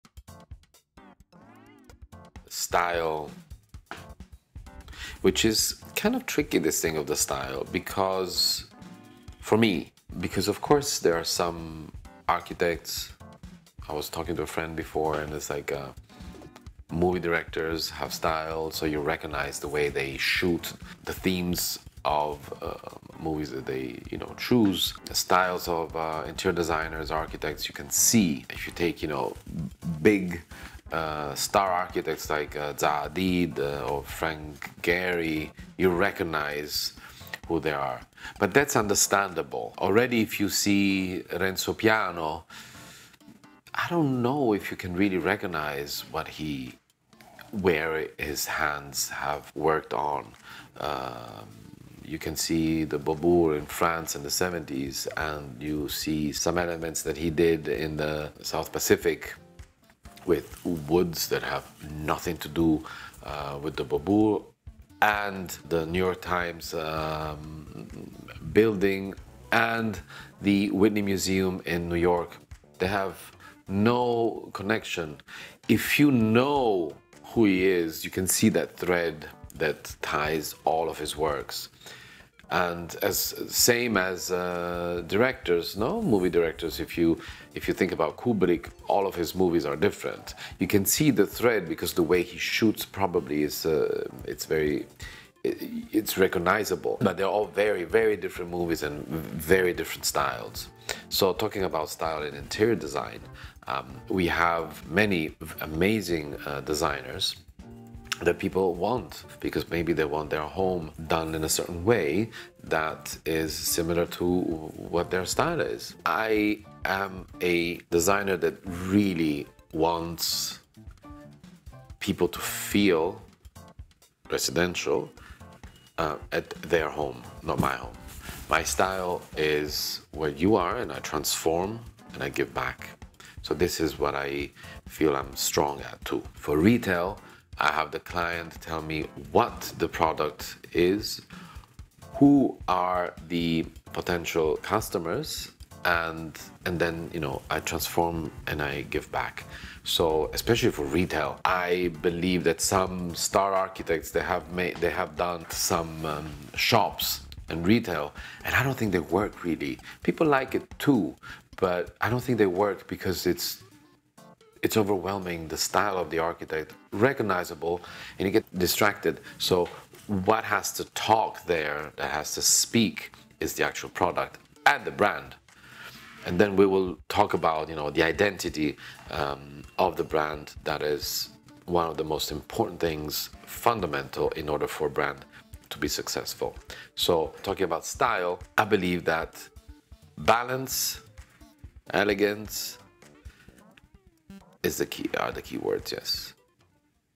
What Is A Style? Starchitects and...
I am trying to explain the difference between a recognizable style and your own personal and unique style. Sometimes there is a confusion where there is the need to be recognized by a style or something thatg is widely recognized. The importance is that it is recognized by you or to whom that style belongs.